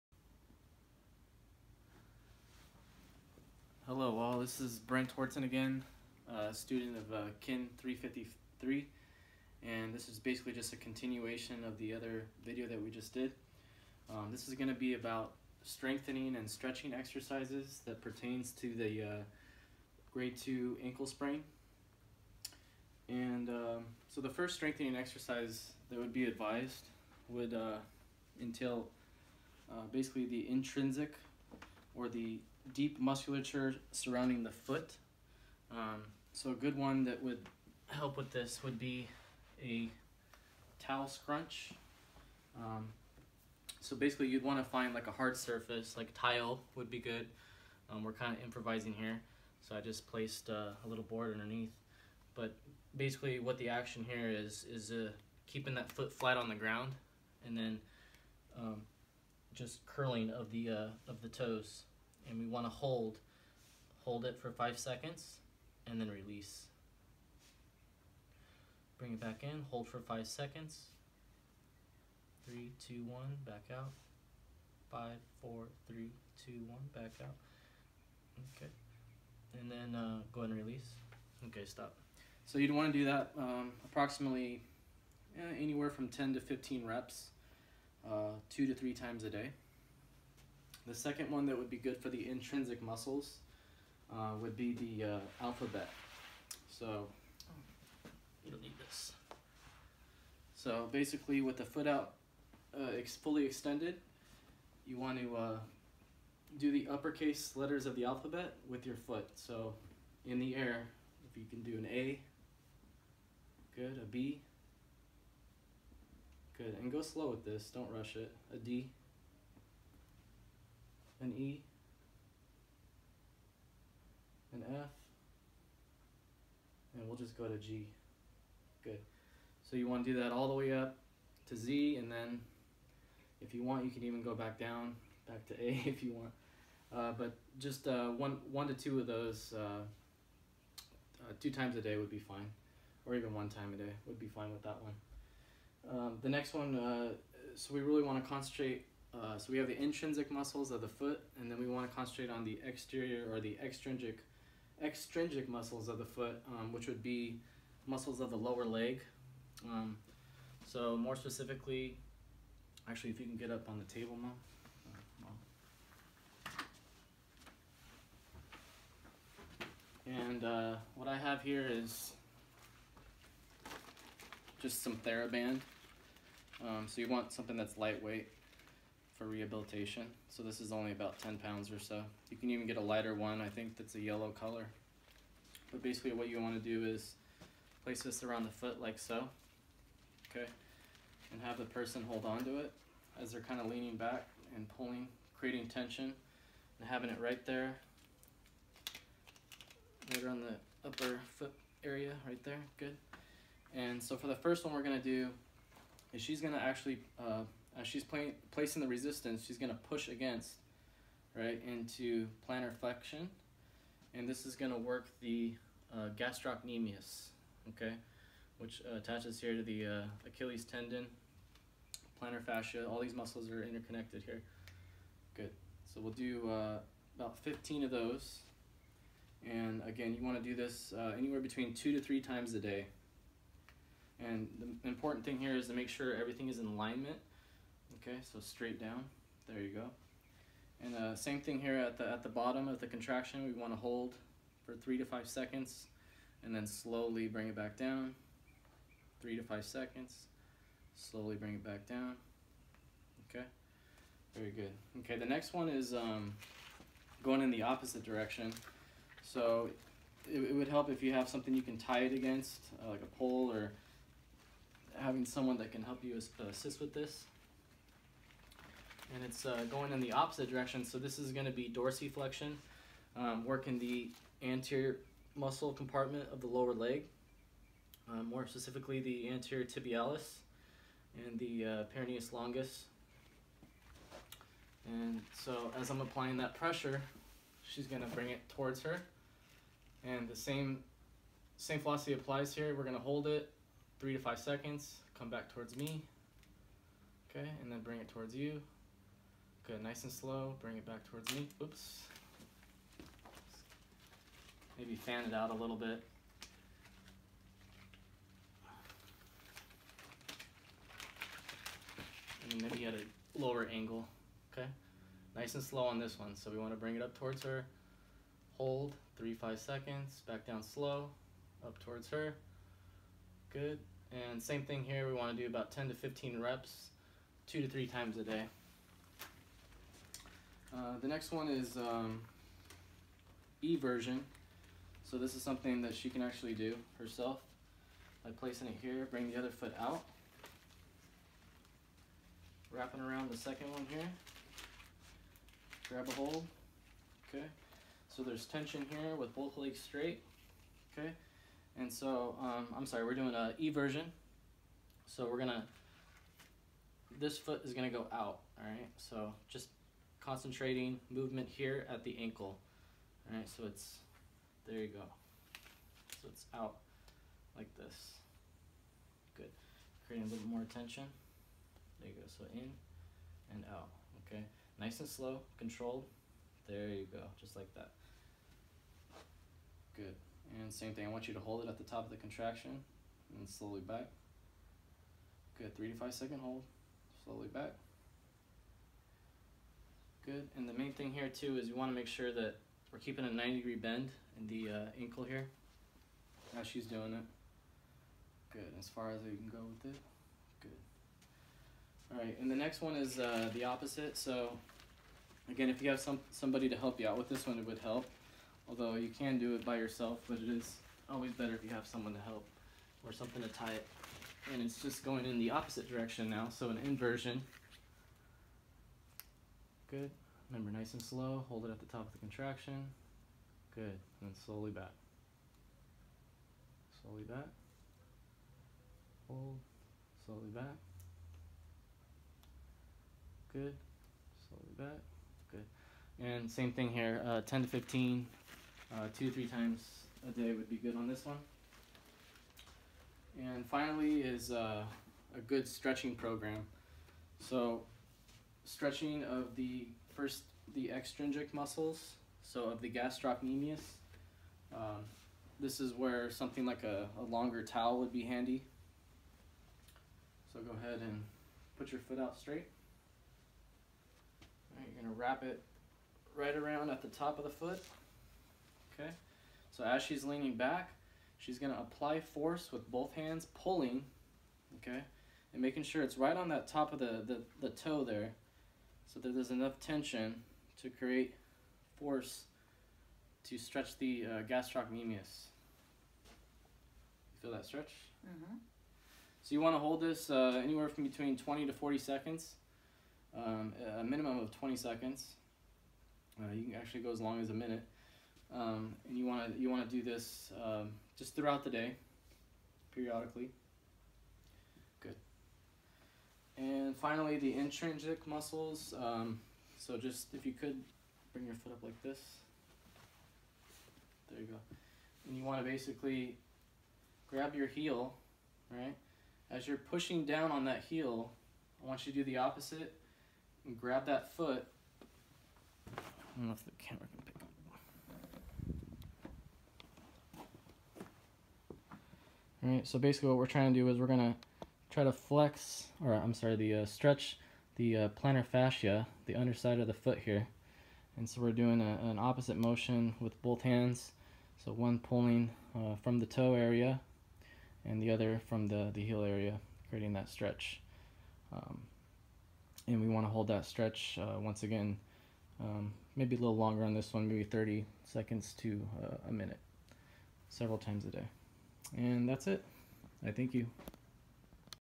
KIN353 Ankle Sprain Exercises
In this video I go over the appropriate exercise and stretching protocol for a grade 2 ankle sprain upon the patient’s ability after pain subsides. I apologize this video couldn't be upload in HQ/HD. Problems with Iphone/Internet connection.